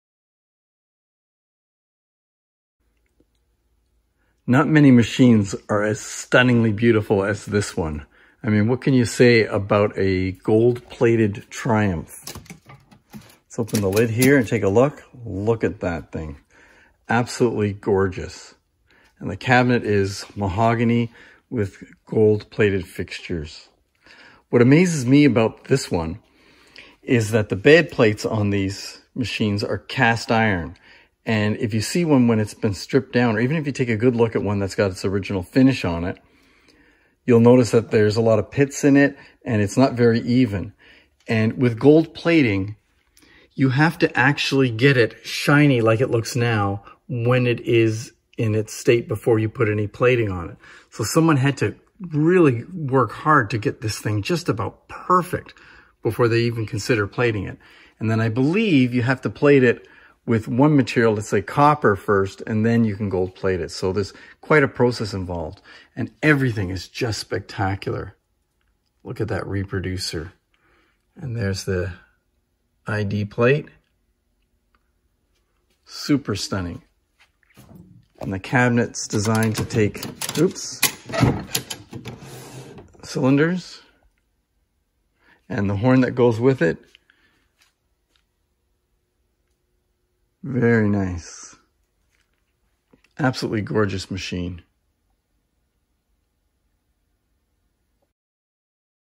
Gold Edison Triumph Phonograph in Hawthorne and Sheble Cabinet
These videos are to showcase some of the rare machines in the Domenic DiBernardo Phonograph, Gramophone & Graphophone collection